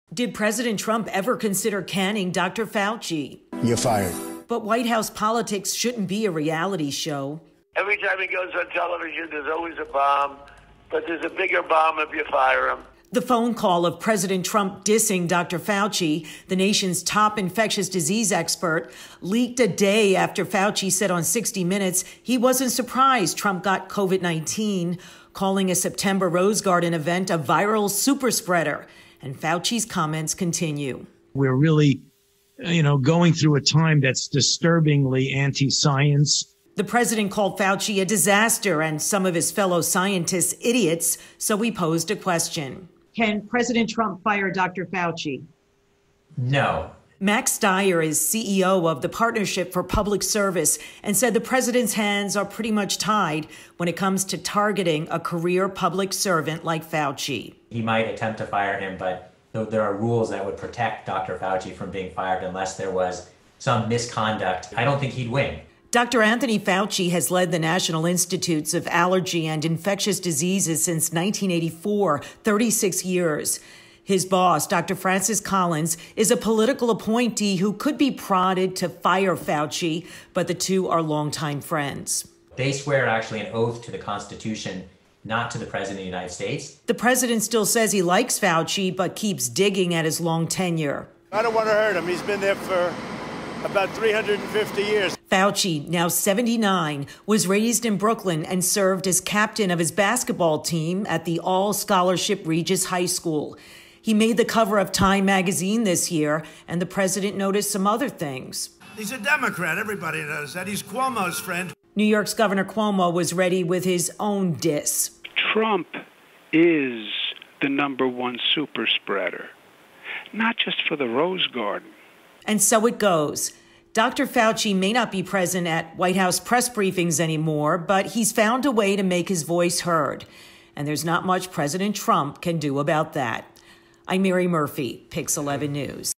Can Pres. Trump fire Dr. Fauci?
President Trump, back on the trail after his own coronavirus hospitalization, blasted government scientists for their criticism of his handling of the pandemic and called Dr. Fauci “a disaster.”